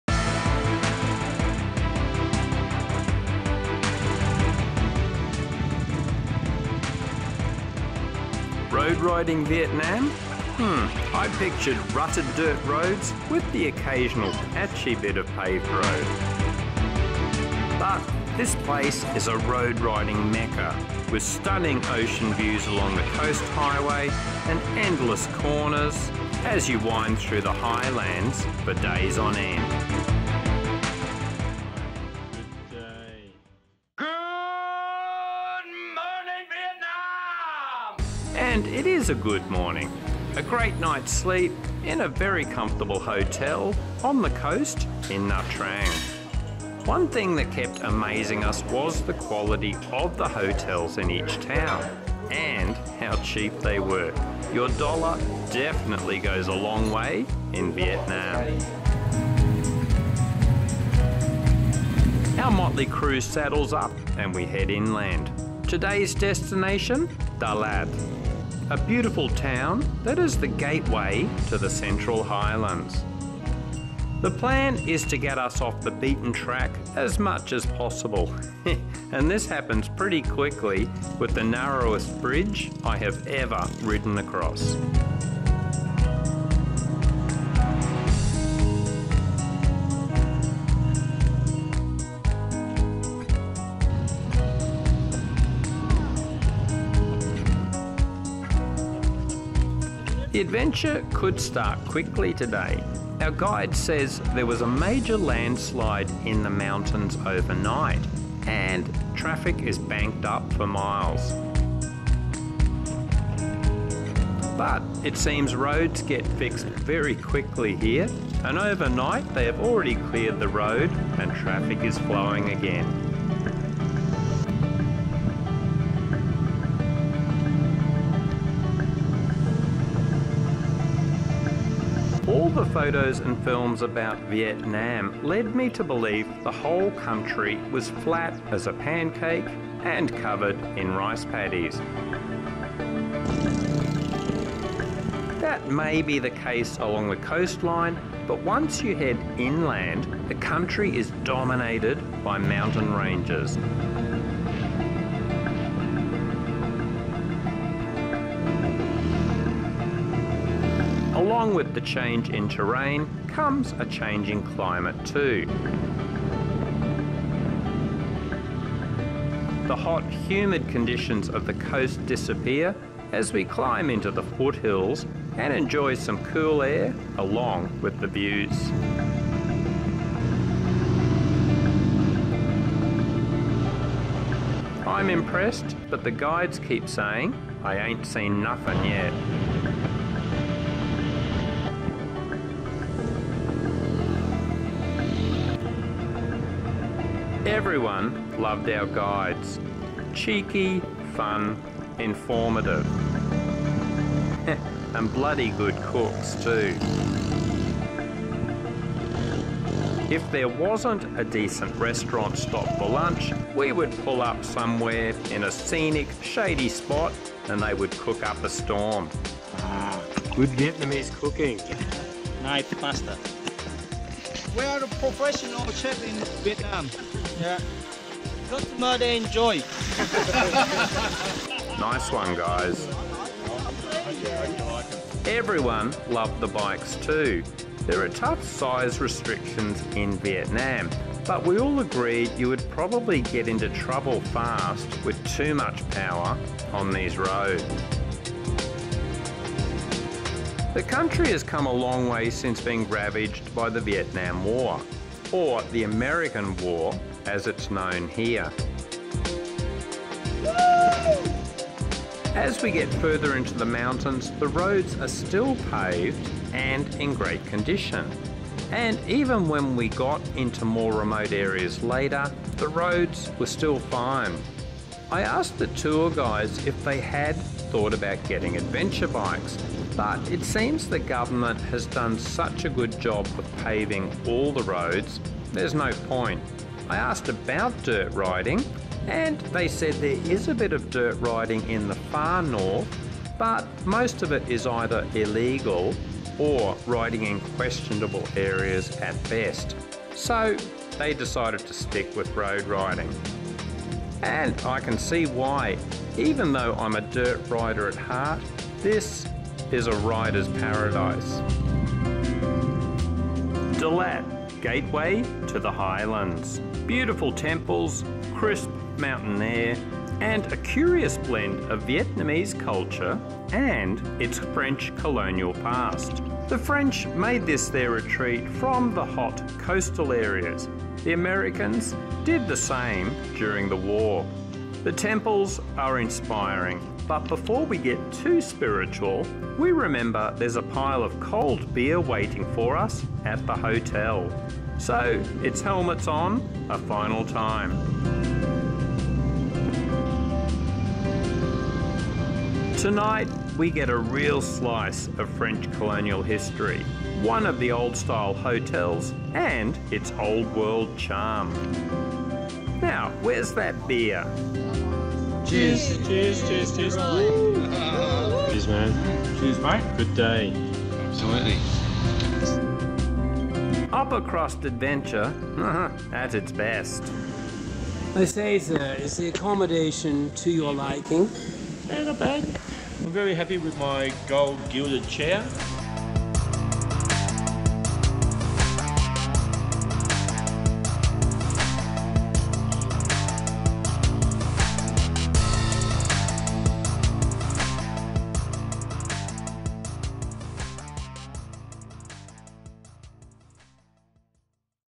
Motorbike riding Vietnam's central highlands︱Cross Training Adventure
Road riding Vietnam? I pictured rutted dirt roads with the occasional patchy bit of paved road but this place is a road riding mecca, with stunning ocean views along the coast highway, and endless corners as you wind through the highlands for days on end with Vietnam Motorbike Tours.

And it is a good morning... a great night's sleep in a very comfortable hotel on the coast in Nha Trang. One thing that kept amazing us was the quality of the hotels in each town and how cheap they were. Your dollar definitely goes a long way with Vietnam Motorbike Tours.

Our motley crew saddles up and we head inland. Today's destination? Dalat. A beautiful Vietnam town that is the gateway to the central highlands. The plan is to get us off the beaten track as much as possible, and it happens pretty quickly with the narrowest bridge I have ever ridden across.

The adventure with Vietnam Motorbike Tours could start quickly today. Our guide says there was a major landslide in the mountains overnight and traffic is banked up for miles. But apparently roads get fixed very quickly here and overnight they have already cleared the road and traffic is flowing again.

All the photos and films about Vietnam led me to believe the whole country was flat as a pancake and covered in rice paddies. That tends to be the case along the coastline, but once you head inland the country is dominated by mountain ranges. Along with the change in terrain comes a change in climate too and hot humid conditions of the coast disappear as we climb into the foothills and enjoy cool air along with the views. I'm impressed but the guides say I ain't seen nothing yet.

Everyone loved our Vietnam Motorbike Tours guides. Cheeky, fun, informative. And good cooks too. If there wasn't a decent restaurant stop for lunch we would pull up somewhere in a scenic shady spot and they would cook up a storm. Nice one guys!

Everyone loved the Vietnam Motorbike Tours bikes too. There are tough size restrictions in Vietnam but we all agreed you would probably get into trouble fast with too much power on these roads.
Vietnam has come a long way since being ravaged by the Vietnam War... or the American was as it's known here. As we get further into the mountains the roads are still paved and in great condition. And even when we get into remote areas later on the roads are still fine. I asked the tour guys if they had thought about getting adventure bikes but it seems the government has done such a good job of paving all the roads that there's no point. I asked about Vietnam dirt riding and they said there is a bit of dirt riding in the far north but most of it is either illegal or riding in questionable areas so they have stuck with the road riding. And I can see why with Vietnam Motorbike Tours. Even though I'm a dirt rider at heart this is a rider's paradise.

Dalat, gateway to the Vietnam highlands. Beautiful temples, crisp mountain air, and a curious blend of Vietnamese culture and its French colonial past. The French colonialists made this their retreat from the hot coastal areas, the Americans did the same thing during the war.

The Vietnam temples are inspiring, but before we get too spiritual we remember there's a pile of cold beer waiting for us at the hotel so it's helmets on a final time. Tonight we get a real slice of French colonial history... one of the old style hotels and its old world charm. Now where's that beer?
Upper crust adventure at its best, Vietnam Motorbike Tours.